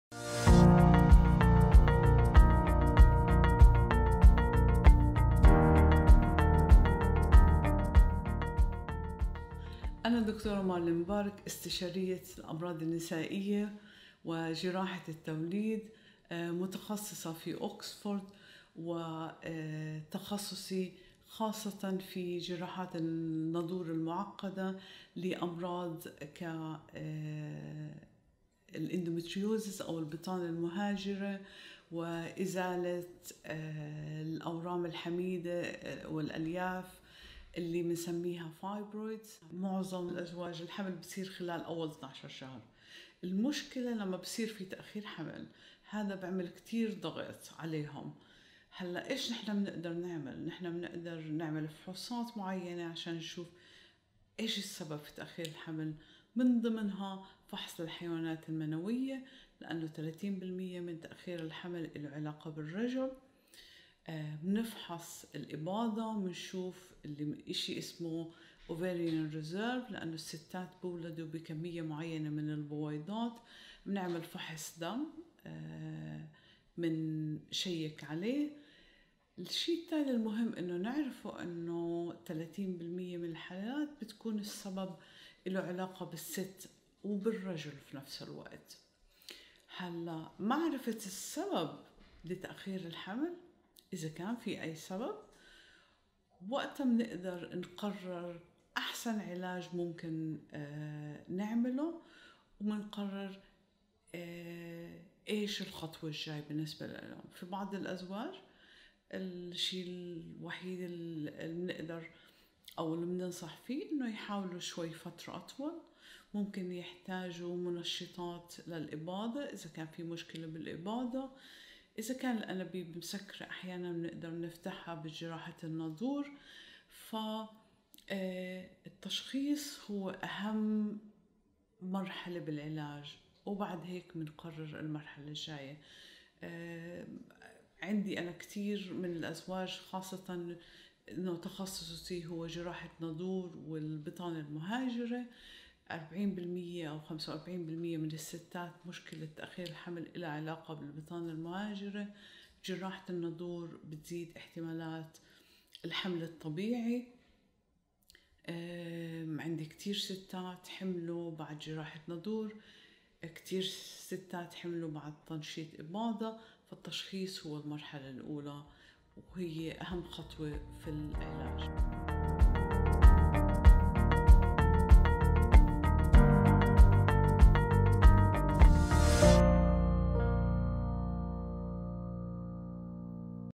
فحوصات الخصوبة في مستشفى كينغز كوليدج في دبي
لمعرفة أسباب تأخر الحمل يمكننا اجراء فحوصات للرجل و المرأة للتشخيص و توفير العلاج الأنسب لكل حالة. شاهد الدكتورة مارلين مبارك لمعرفة المزيد عن فحوصات الخصوبة. الدكتورة مارلين مبارك هي استشارية طب النساء و التوليد أكملت تخصصها في جامعة  أكسفورد في المملكة المتحدة، و تختص بجراحات الناظور المعقدة و خاصة البطانة المهاجرة.
للاستفسار و حجز موعد، اتصل على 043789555.